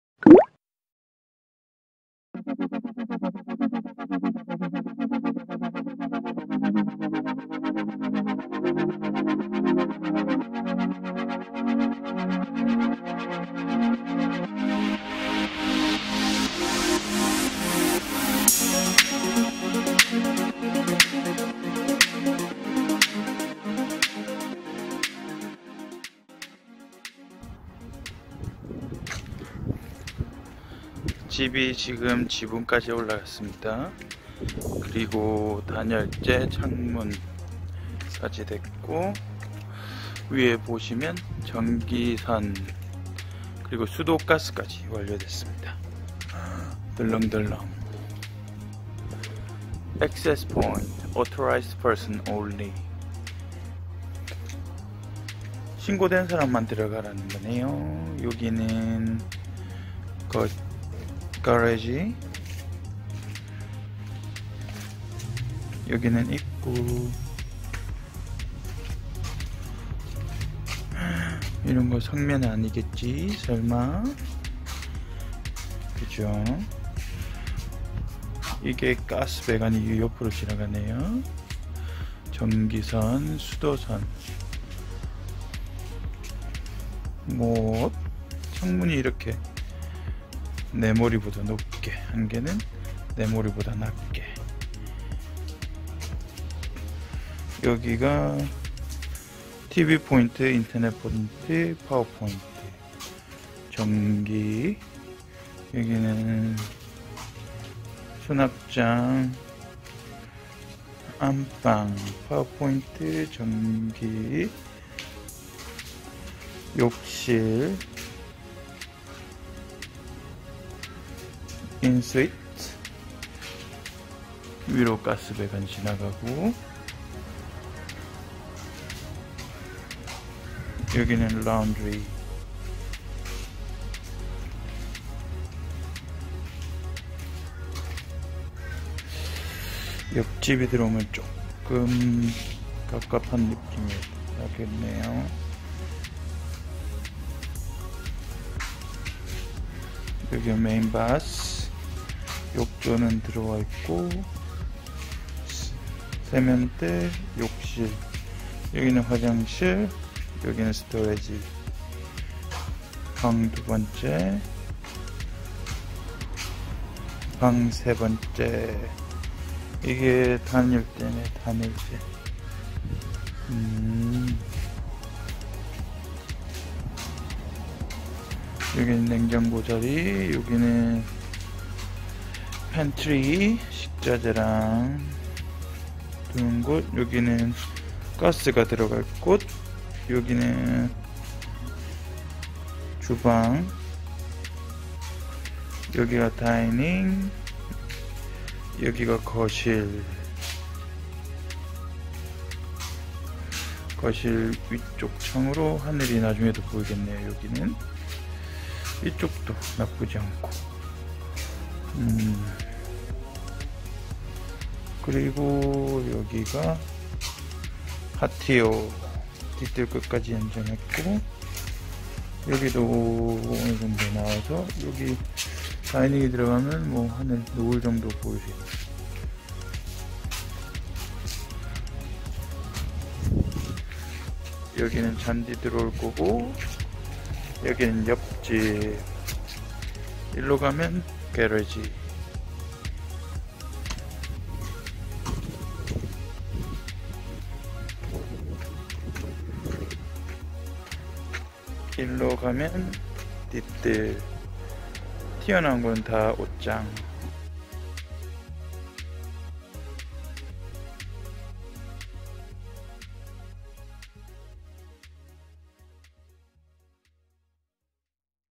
[Suby's 호주이야기] 내집짓기:프롤로그 - 호주 이민 1년차 땅사서 집짓기
호주 주택의 구조 및 특징
집짓는데 걸리는 시간
집 지어지는 과정